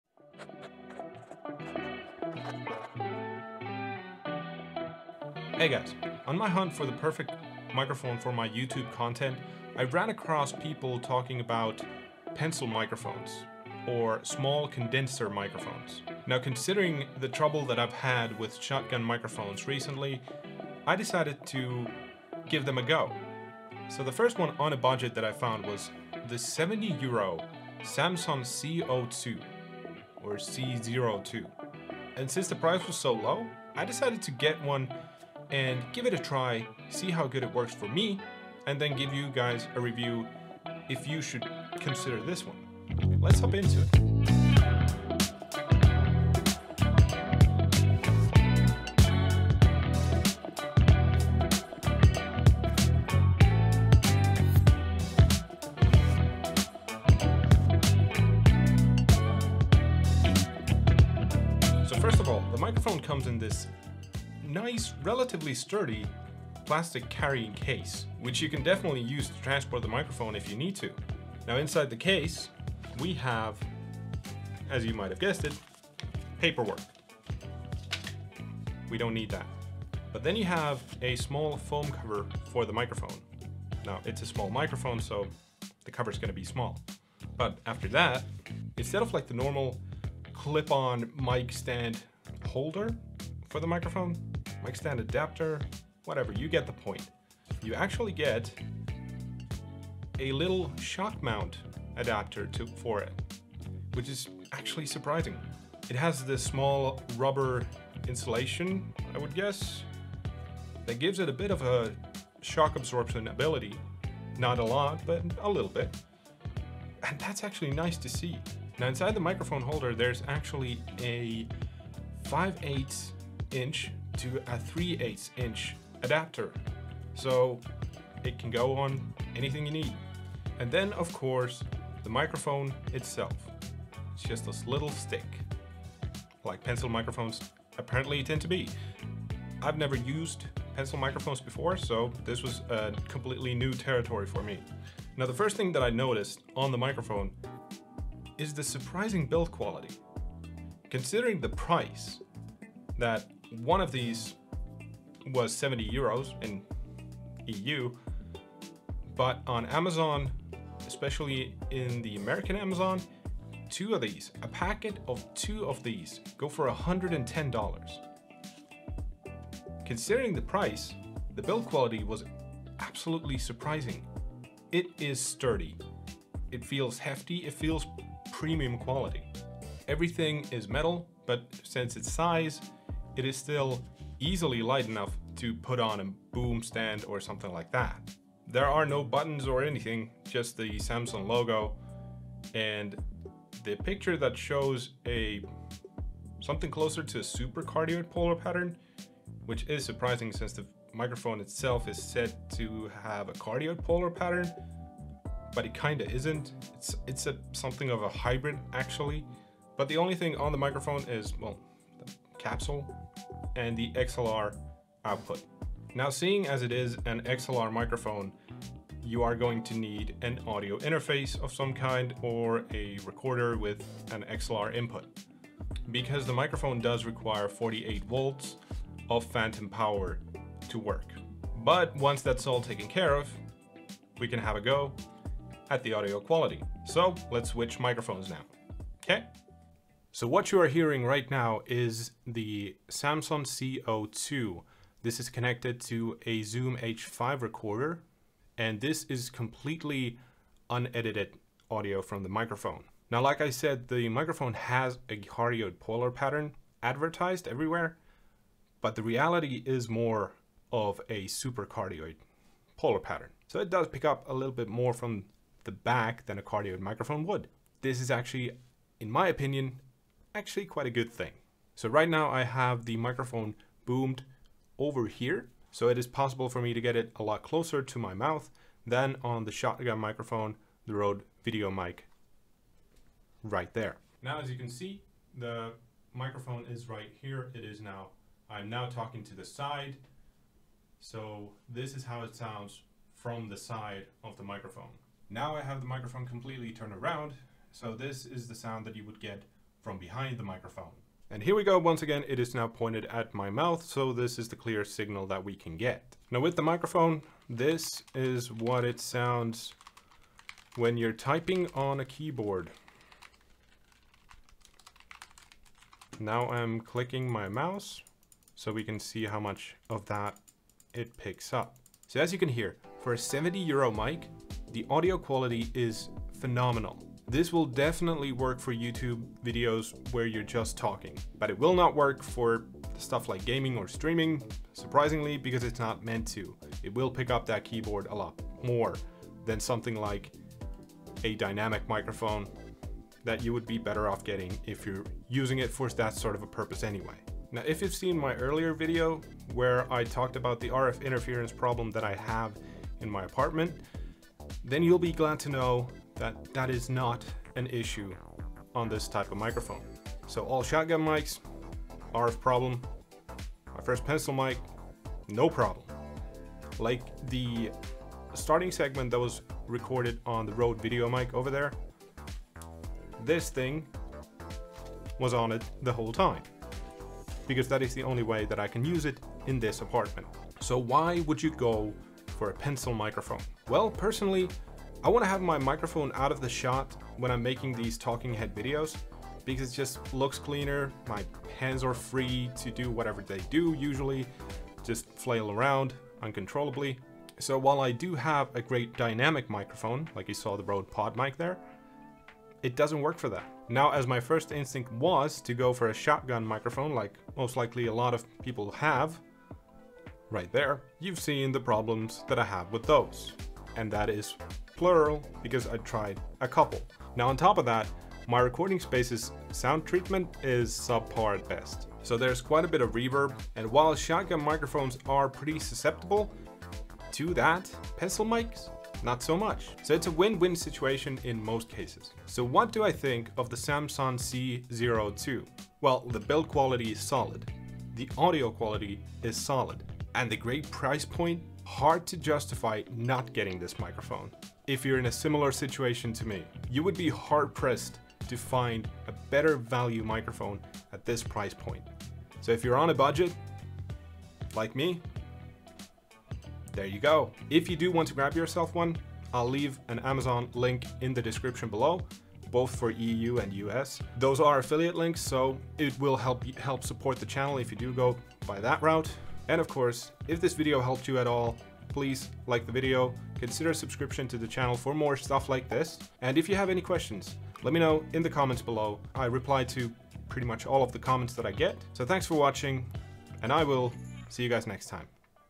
Is This The Best Budget Mic For Youtube? - Samson C02 Review
My quest for the best budget mic for indoor dialogue has reached it's end for the foreseeable future. It doesn't get better than this! Check it out!

Get your mics!
U.S (DOUBLE Pack)
EU (SINGLE Mic)

Socials!